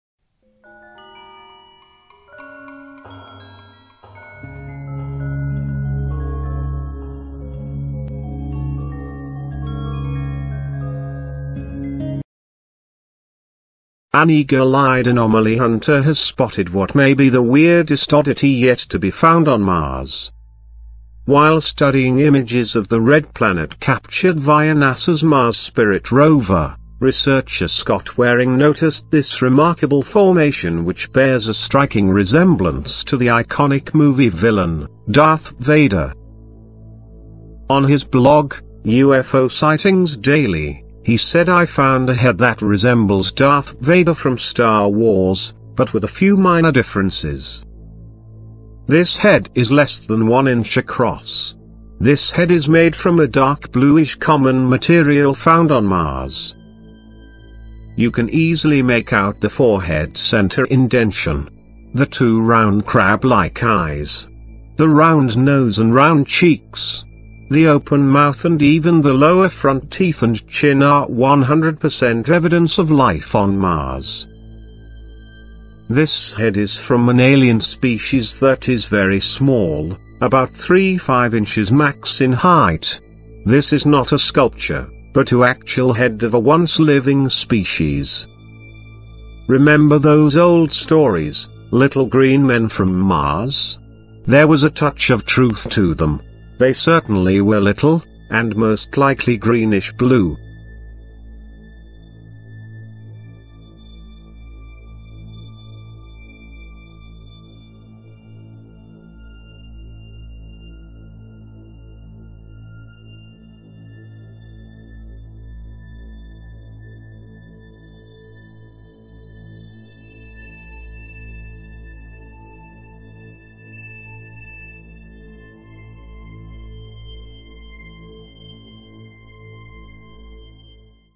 Alien Head Resembling Darth Vader Found On Mars
An eagle-eyed anomaly hunter has spotted what may be the weirdest oddity yet to be found on Mars.

While studying images of the Red Planet captured via NASA's Mars Spirit Rover, researcher Scott Waring noticed this remarkable formation which bears a striking resemblance to the iconic movie villain Darth Vader.